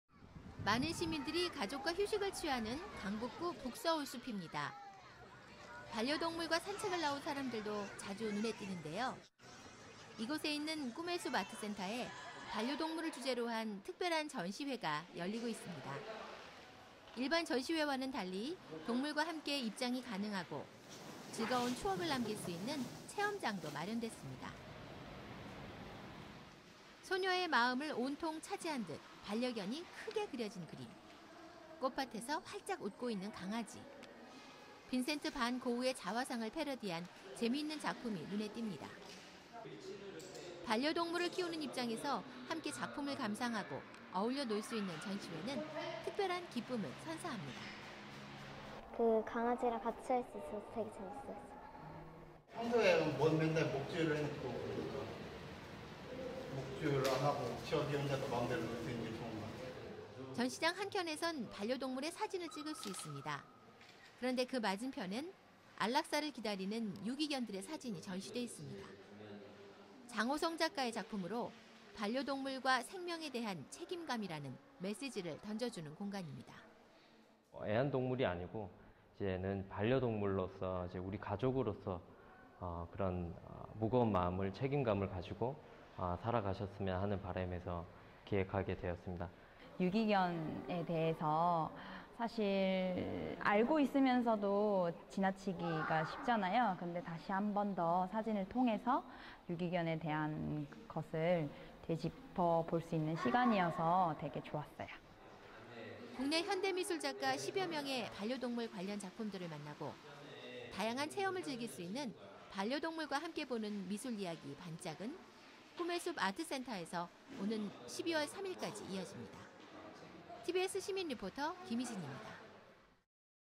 [tbs시민리포트] 반려동물과 함께 즐기는 전시회
우리나라의 반려동물 인구가 1000만명을 넘어서 꾸준히 증가하고 있습니다.
그런데 그와 함께 유기견의 수도 늘고 있죠.

얼마 전,반려동물과 함께 즐길 수 있는 미술전시회가 오픈했는데
다양한 미술작품 감상과 함께
생명에 대한 책임감도 느낄 수 있는 자리가 되고 있습니다.

tbs 시민리포터가 소개합니다.